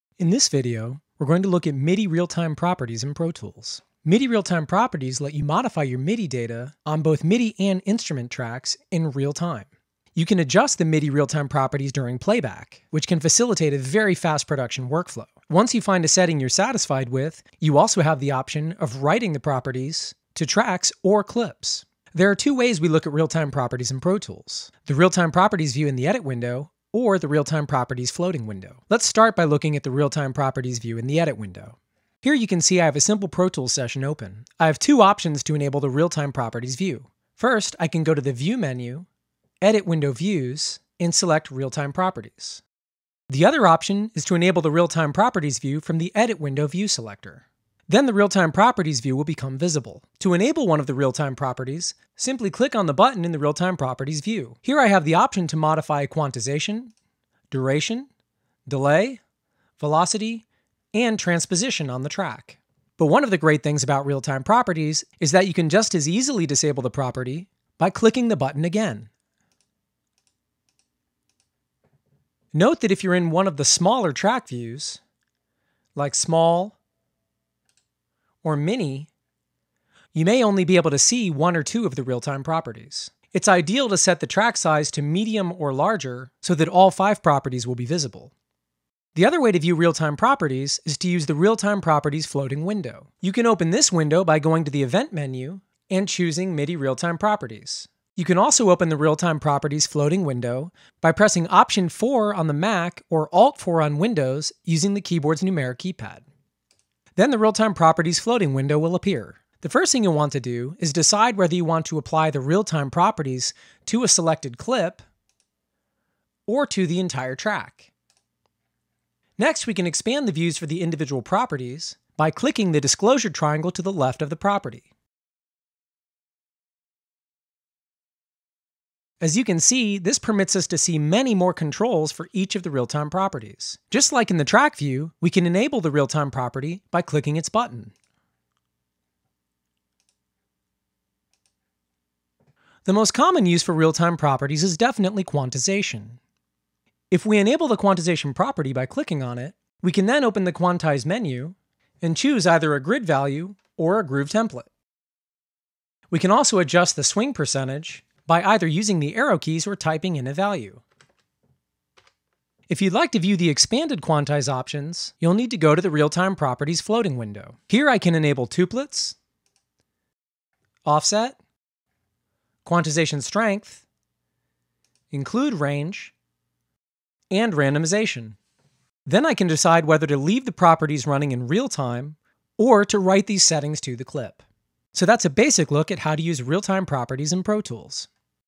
MIDI Real-Time Properties in Pro Tools
An overview of the MIDI Real-Time Properties in Pro Tools..